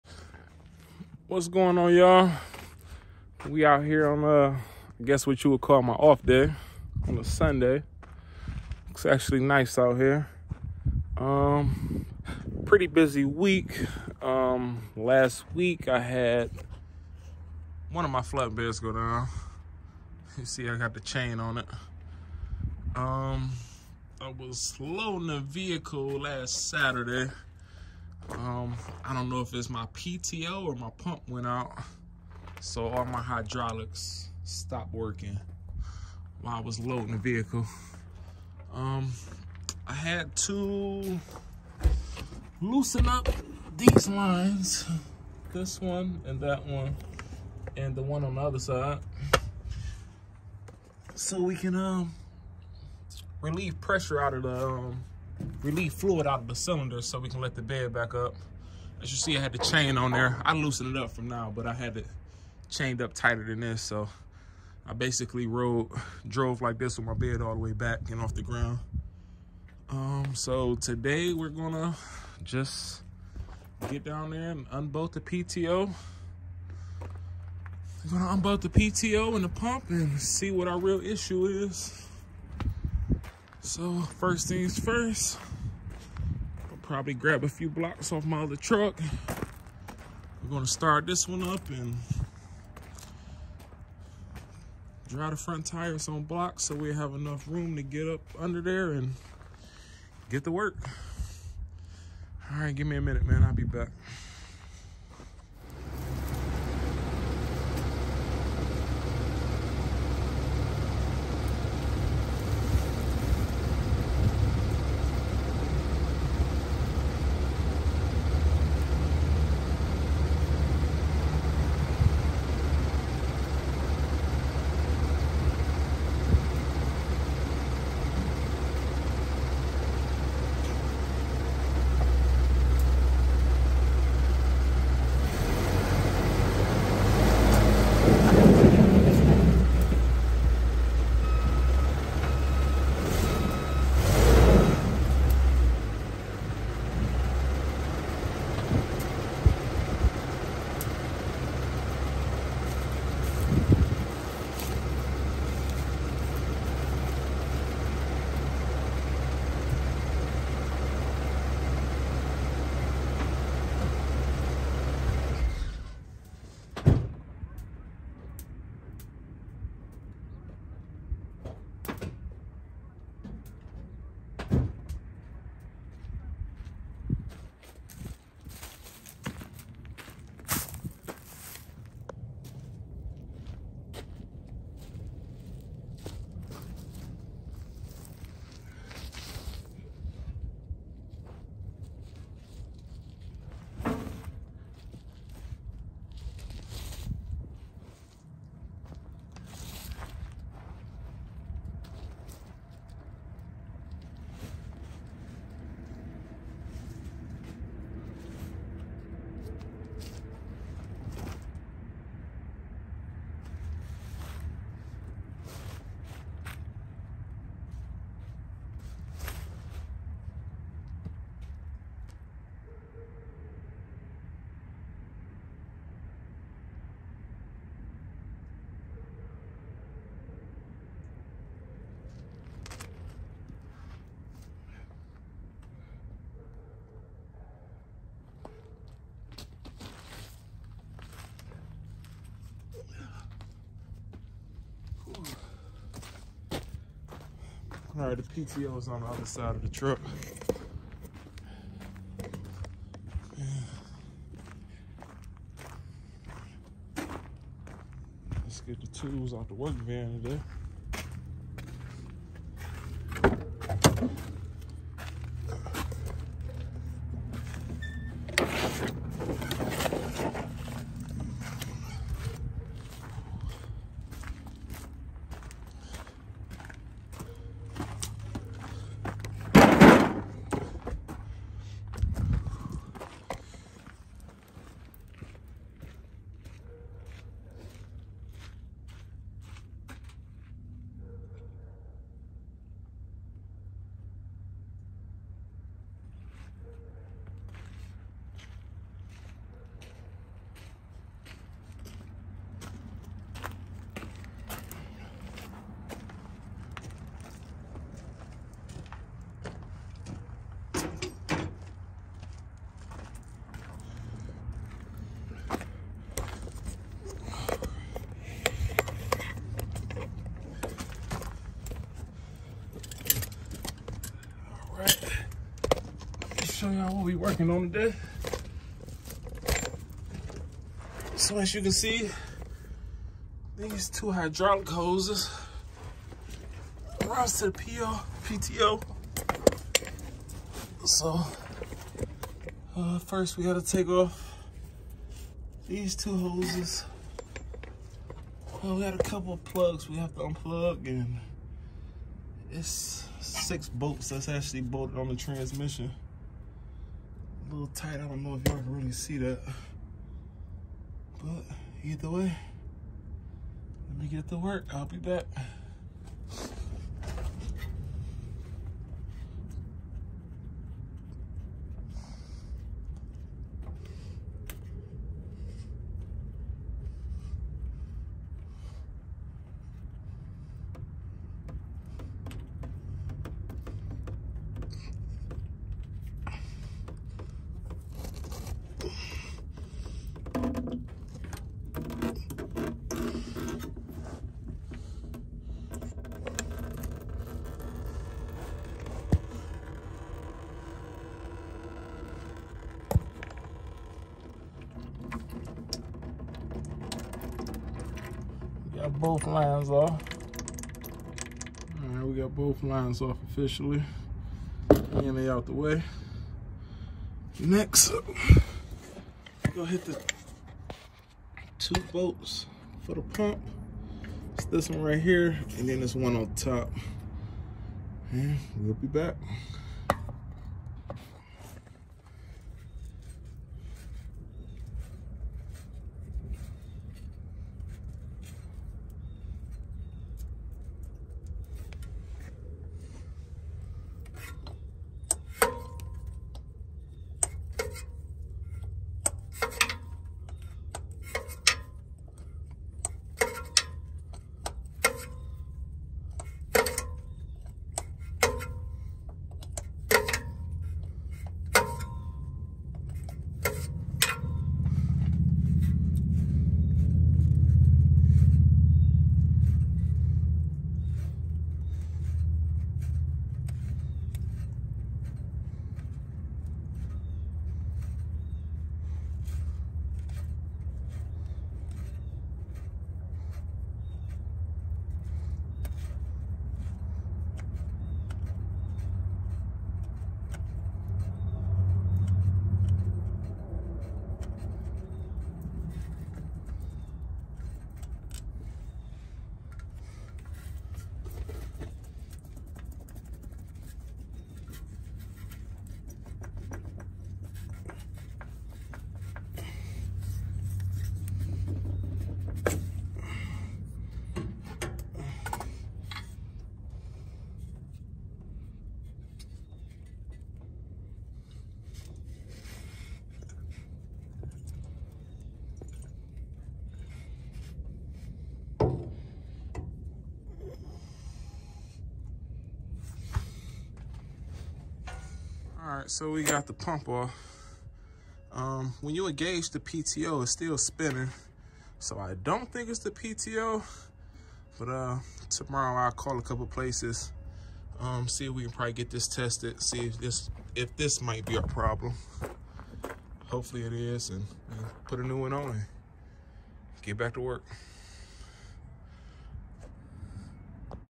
Pulling the hydraulic pump off my tow truck
Pulling the hydraulic pump off my flatbed tow truck. I slowly lost all hydraulic pressure none of my controls worked for the bed or wheel-lift nor the witch.